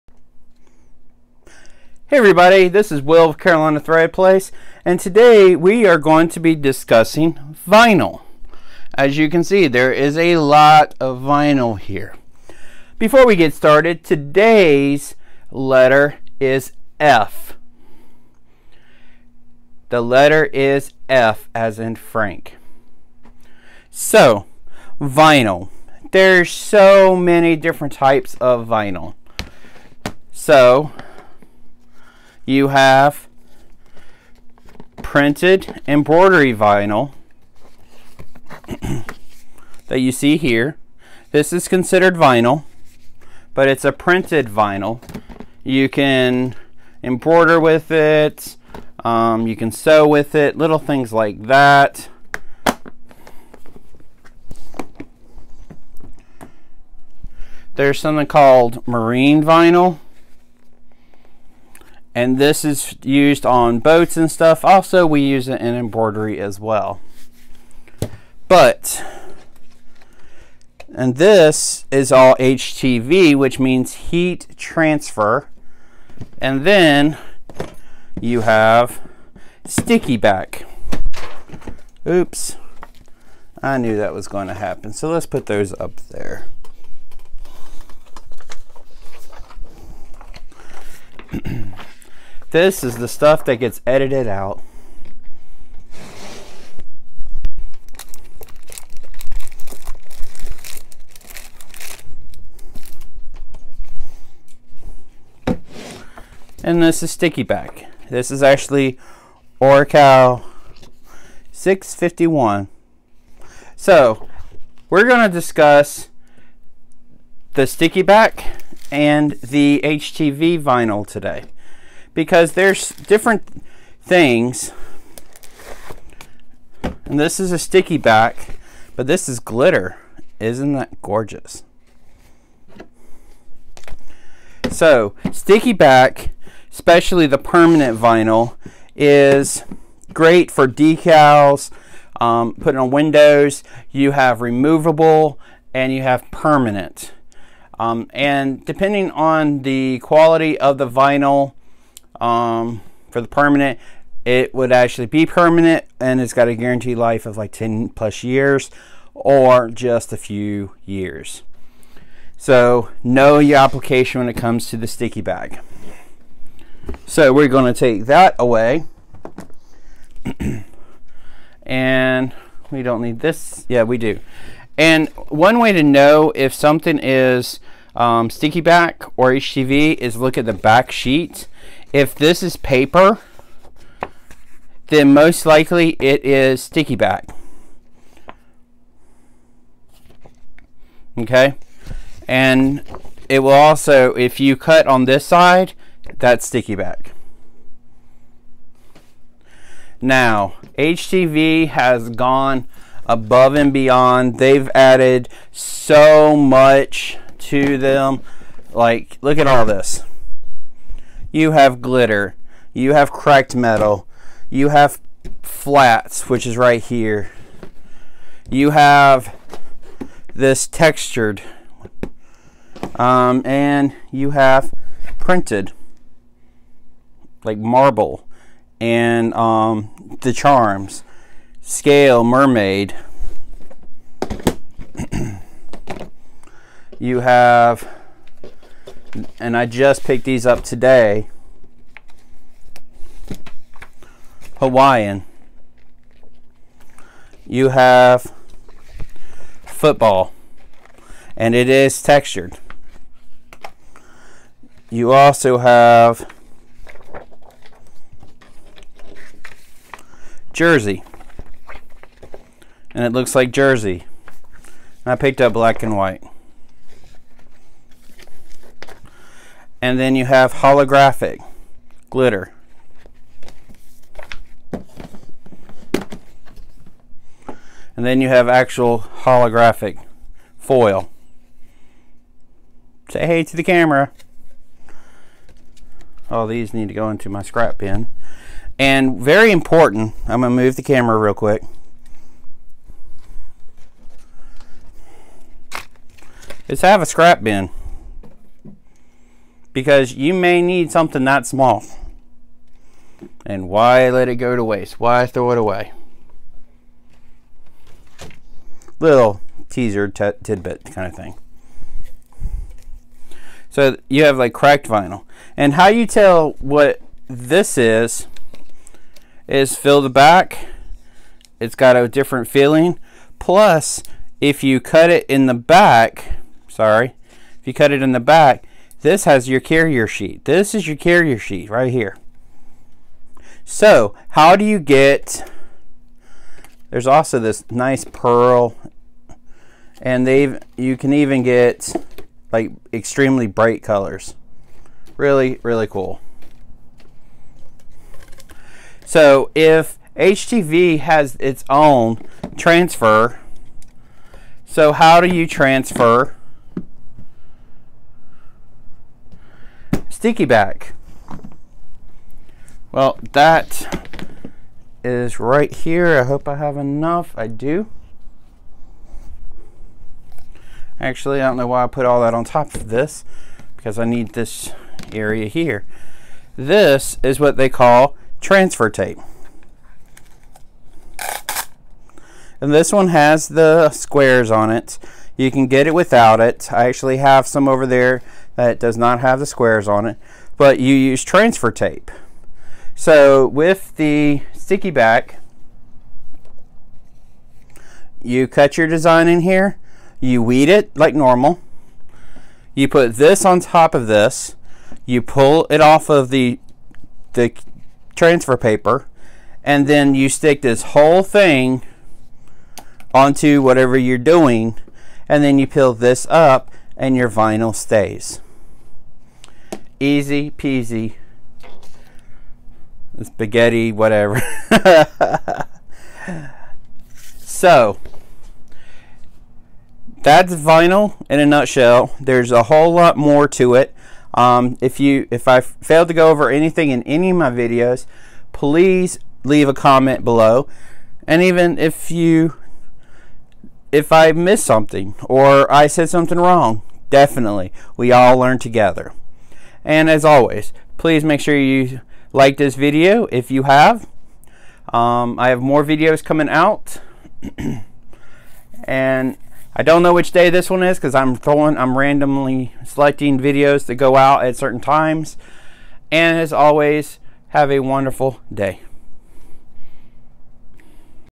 All About Vinyl | Carolina Thread Place
Today we talk about all different types of vinyl and I show you just some of the available vinyl.
6/31 - 25 more to go!